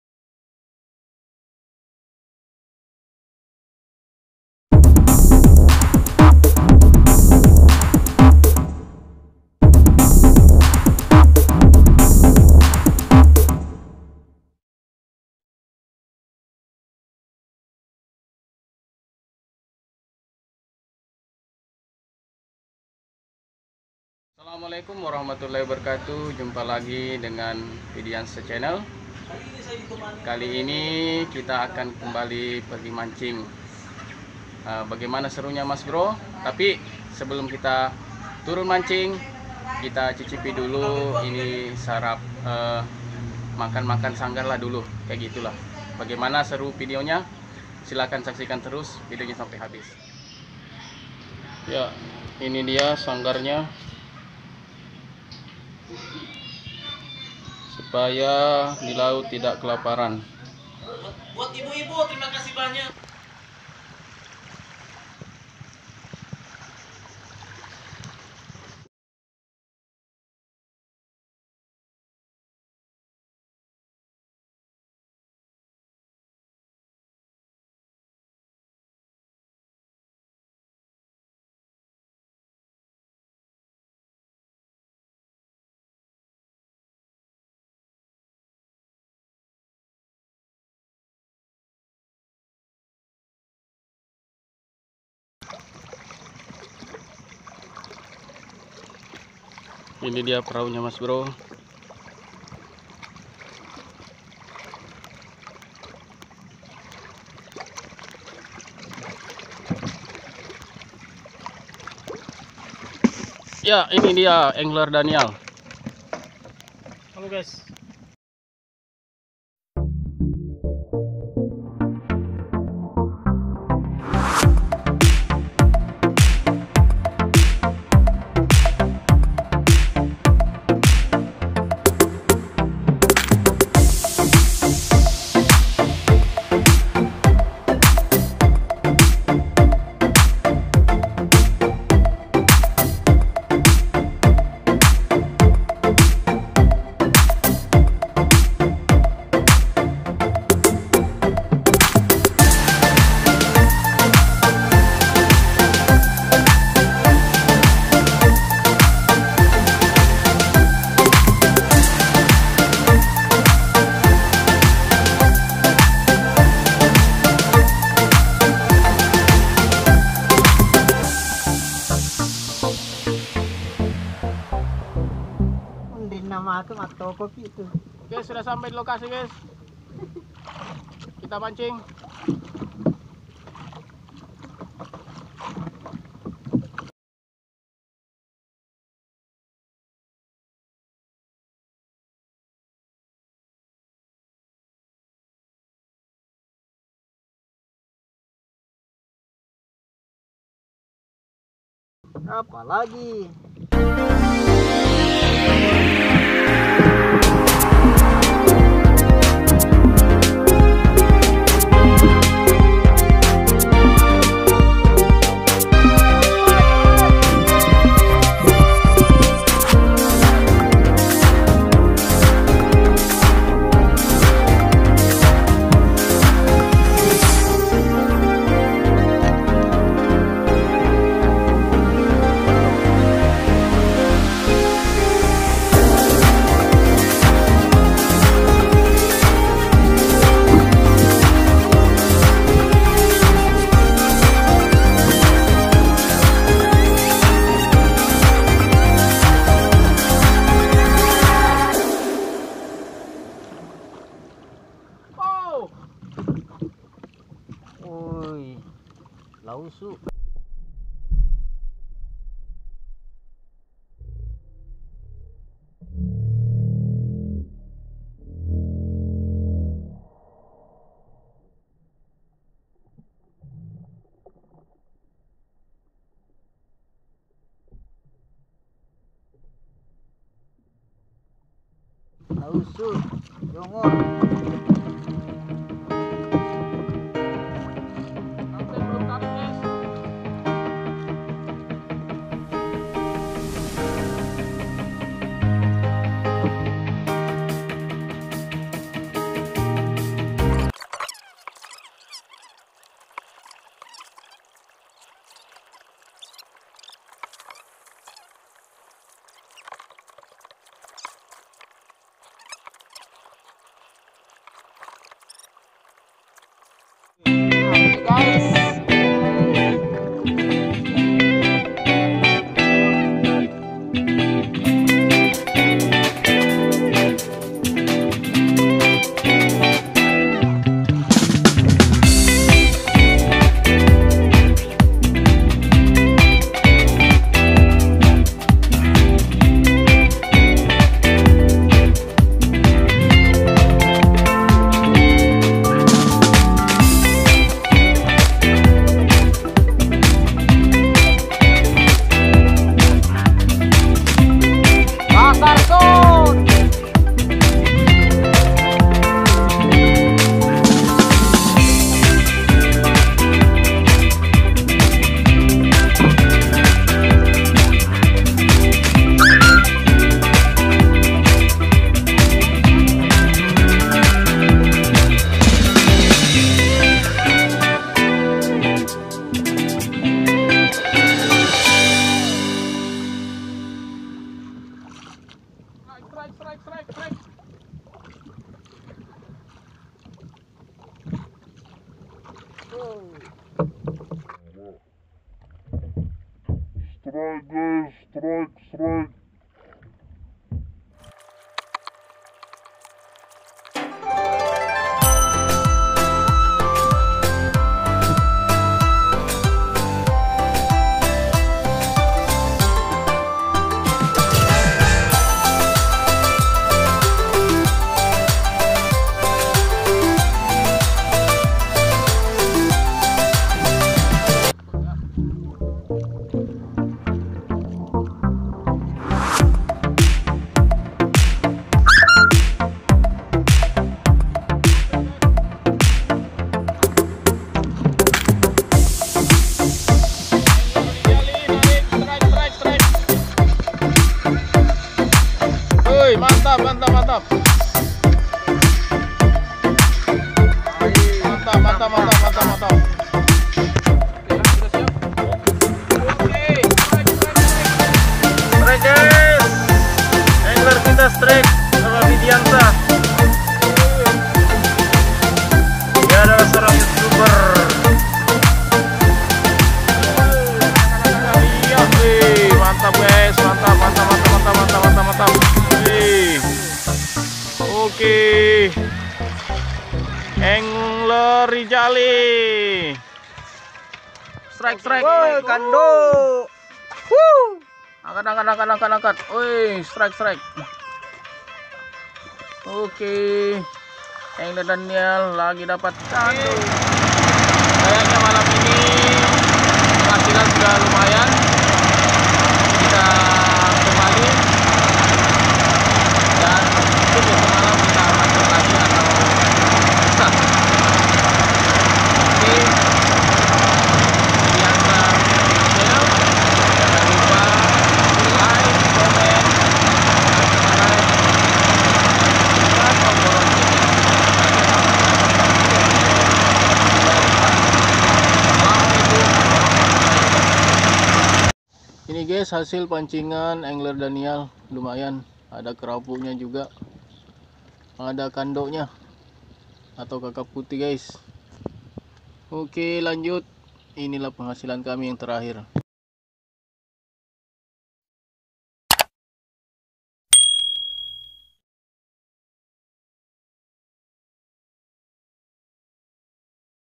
MANCING DILAUT || IKAN DASARAN
Video ini menampilkan kegiatan mancing dilaut bersama engler danial dan engler rizali waw alhamdulillah strike juga...